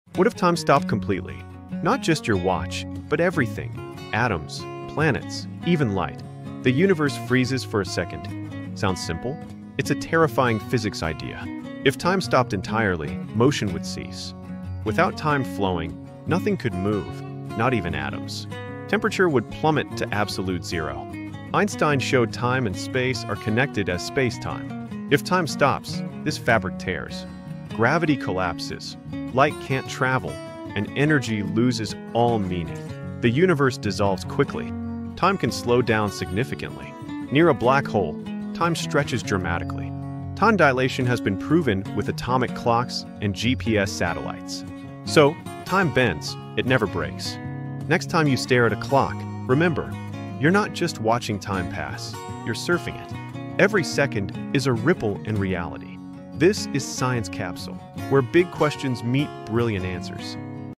What If Time Suddenly Stopped for One Second?#
What if… time suddenly stopped?
Not just your watch — but everything: atoms, planets, even light itself.
For one single second… the universe freezes.
Sounds simple? It’s actually the most terrifying idea in physics.
If time truly stopped, motion would no longer exist.
Because motion *is* time.
Without time flowing, nothing can move — not even the vibration of atoms.
That means *temperature drops to absolute zero*.
Every living cell, every heartbeat, every photon of light… would instantly cease.00:00
What if time stopped completely? Not just your watch, but everything. Atoms, planets, even light. The universe freezes for a second. Sounds simple. It's a terrifying physics idea. If time stopped entirely, motion would cease. Without time flowing, nothing could move, not even atoms. Temperature would plummet to absolute zero. Einstein showed time and space are connected as spacetime. If time stops, this fabric tears, gravity collapses, light can't travel, and energy losses all meaning. The universe dissolves quickly. Time can
00:36

slow down significantly. Near a black hole, time stretches dramatically. Time dilation has been proven with atomic clocks and GPS satellites. So, time bends, it never breaks. Next time you stare at a clock, remember you're not just watching time pass, you're surfing it. Every second is a ripple in reality.